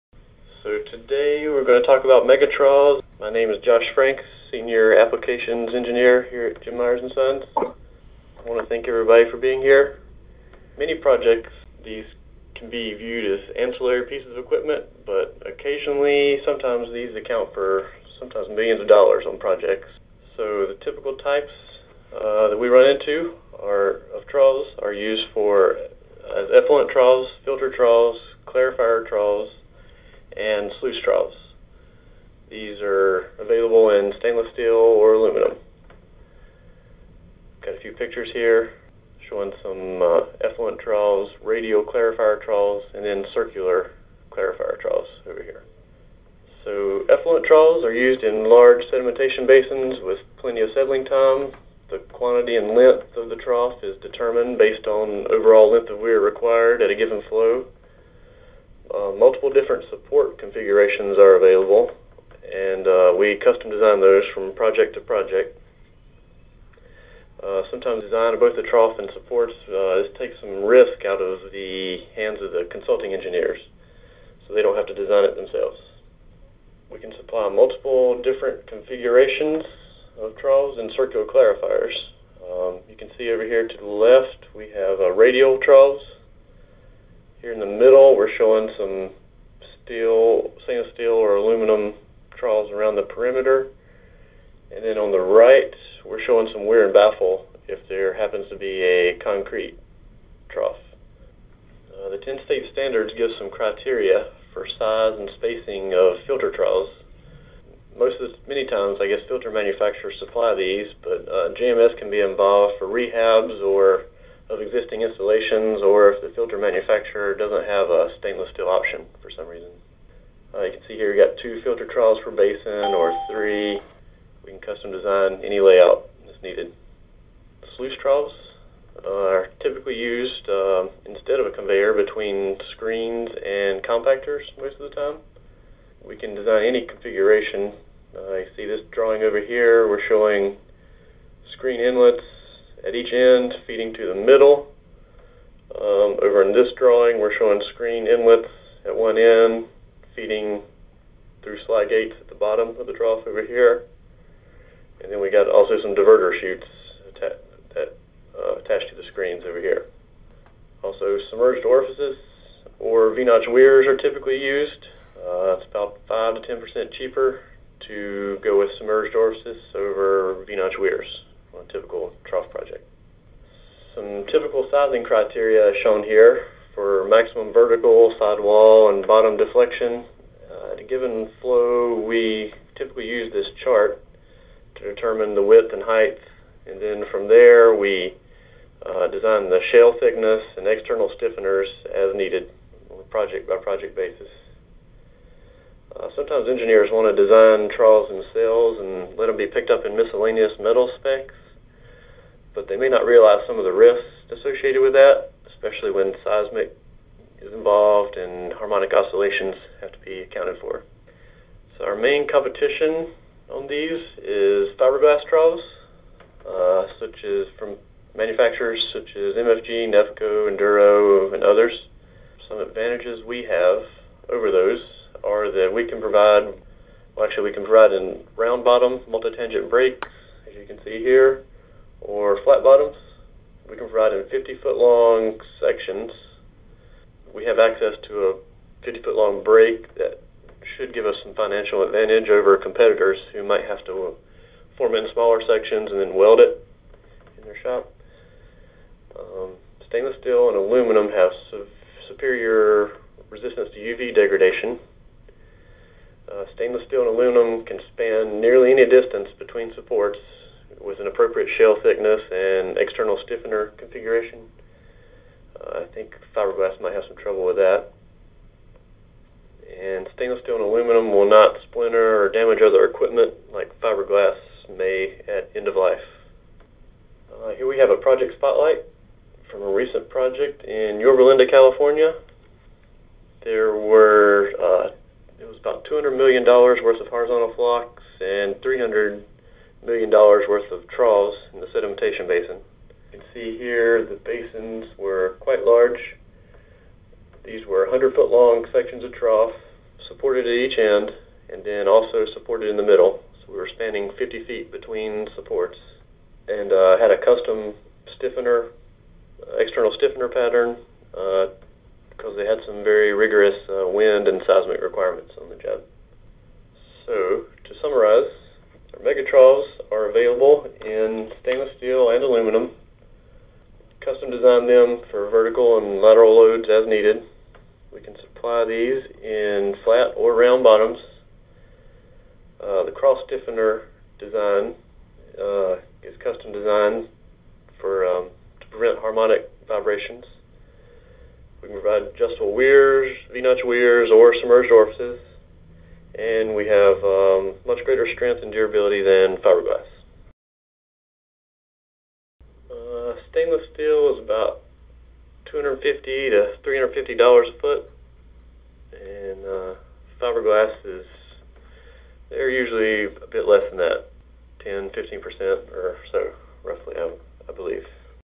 JMS Mega-TROUGH (Trough System) Presentation
JMS Mega-TROUGH (Trough Systems) are custom made in-house for use in both water and wastewater treatment applications. In circular form for use as effluent troughs in clarifiers, and straight orientation for sedimentation basin uses they provide maximum longevity for water management and control. Available in stainless steel or aluminum they can be supplied with either a multi-tangent round bottom, or flat bottom to suit the particular process.